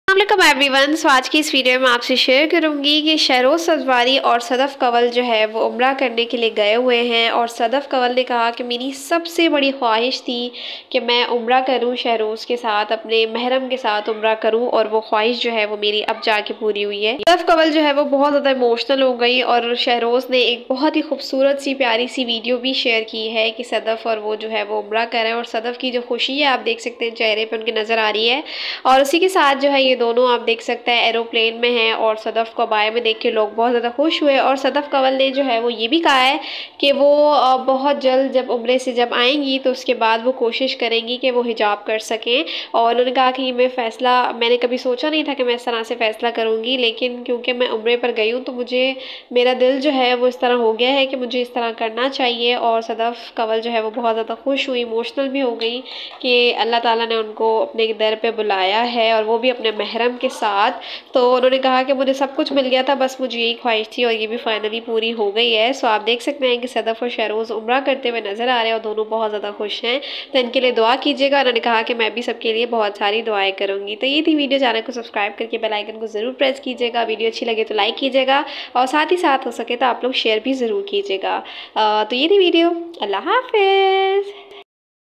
Sadaf kanwal & shahroz sabzwari performing Umrah !! sadaf kanwal emotional 🥺😭
sadaf & shahroz performing Umrah